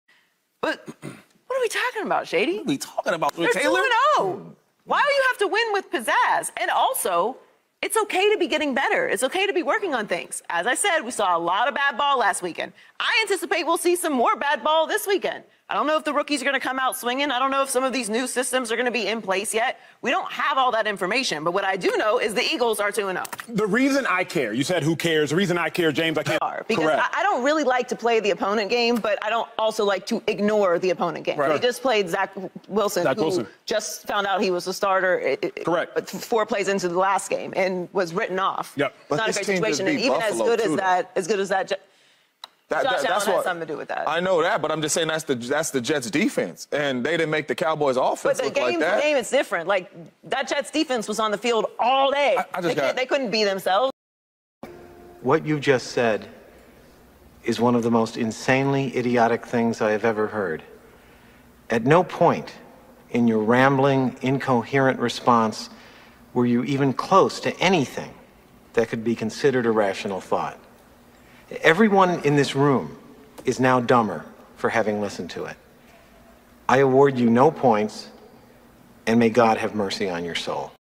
Joy Taylor Exposed
she either paid to try to sell the narrative or had no idea what she's talking about .